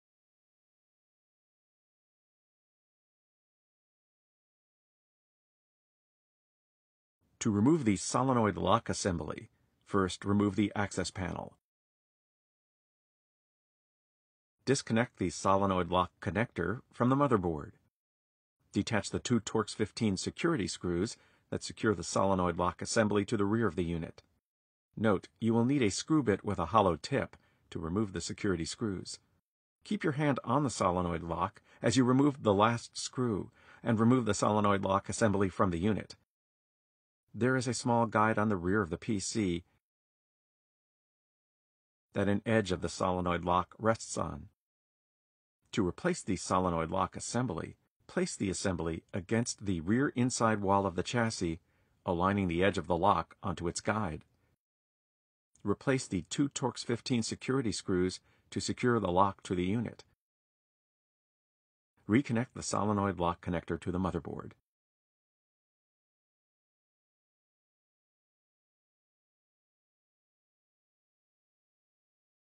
Replace the Solenoid Lock Assembly | HP EliteDesk 705 G2 and G4 Microtower PC
Learn how to remove and replace the solenoid lock assembly for the HP EliteDesk 705 G2 and G4 Microtower PC.